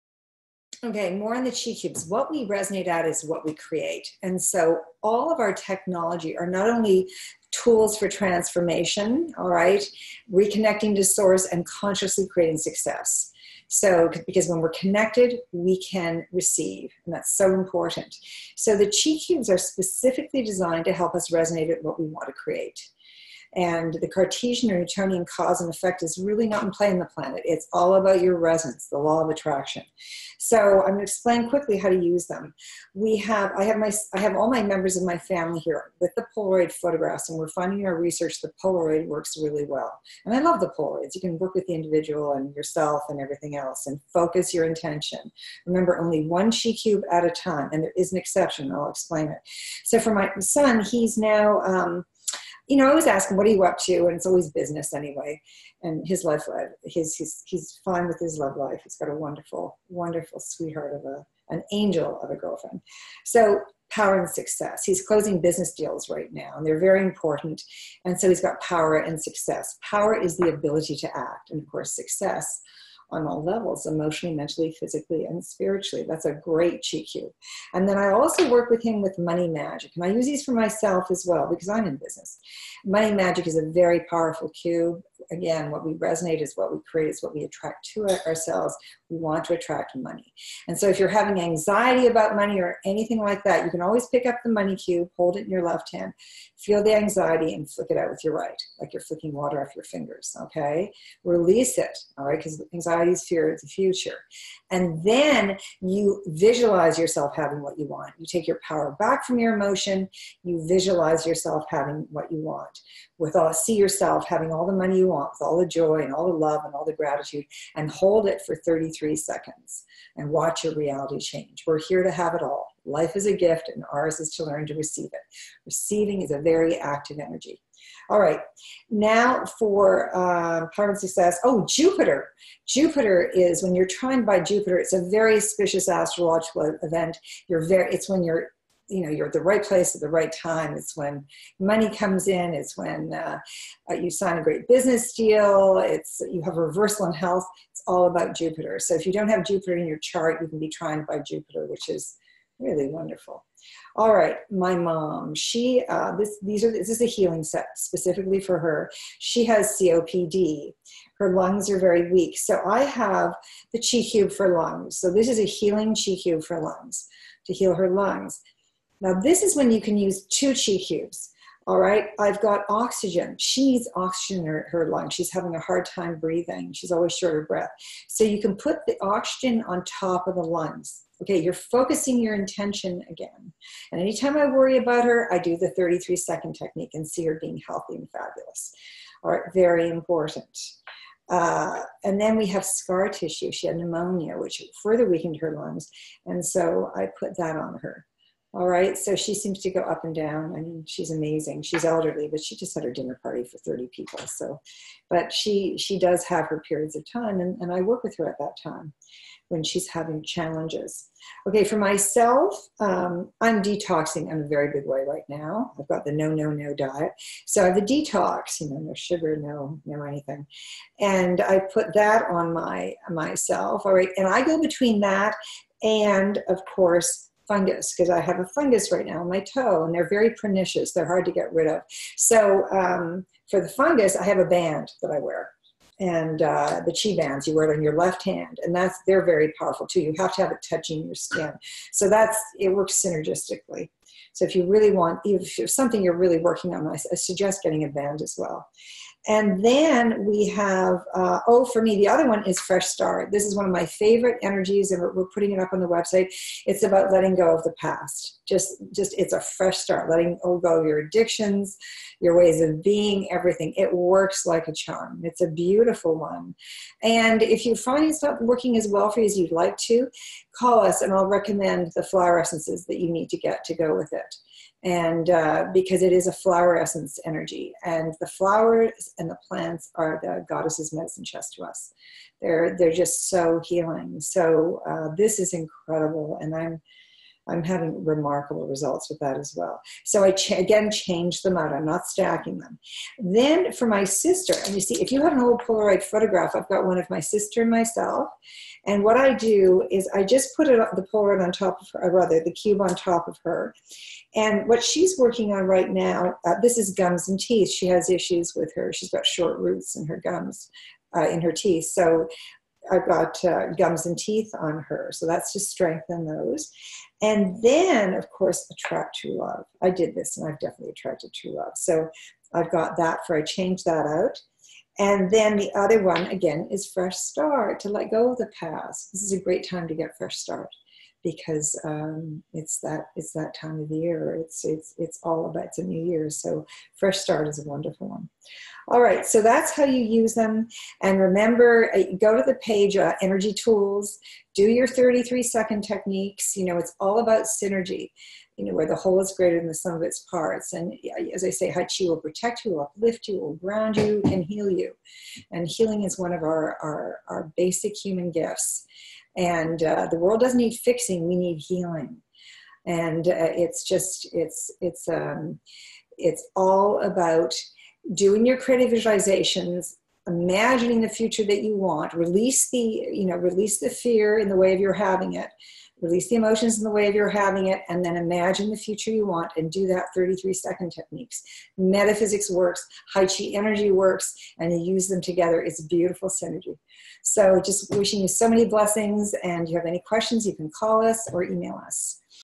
How to use Chi Cubes on Polaroid
Deborah explains how to use the Chi Cubes on a Polaroid.